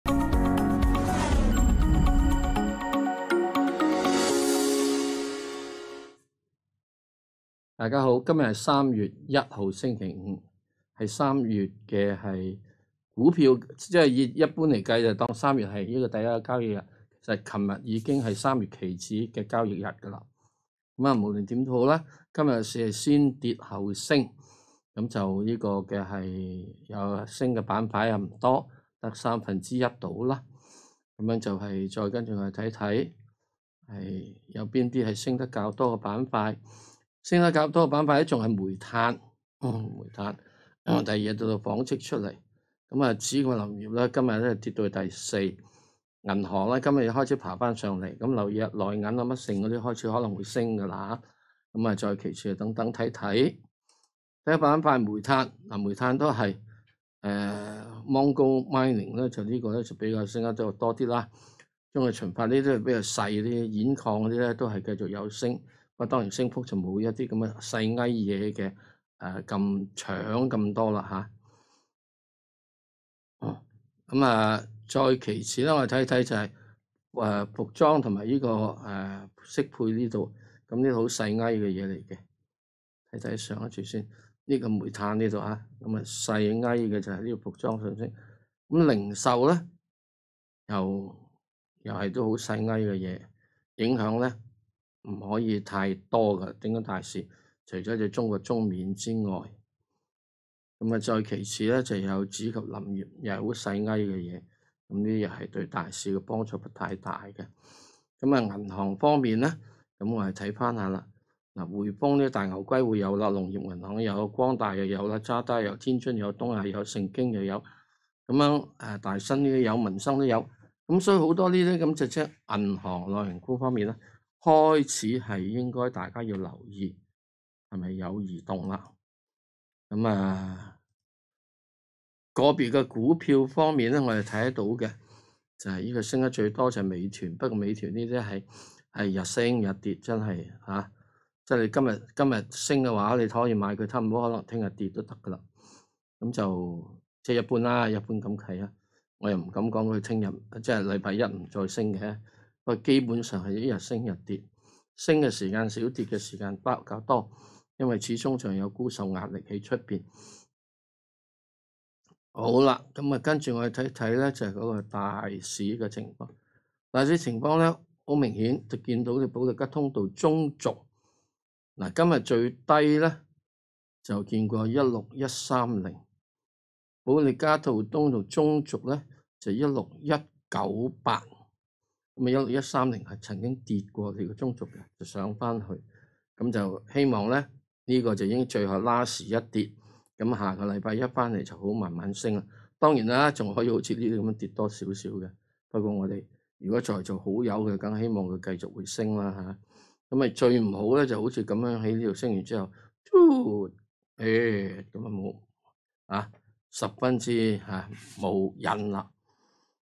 【石Sir午市閒談】今日推介比亞迪電子、敏實集團、喜相逢集團
《香港經濟日報》專欄作者石鏡泉（石Sir）推介比亞迪電子（00285）、敏實集團（00425）、喜相逢集團（02473），詳情待石Sir為你分析。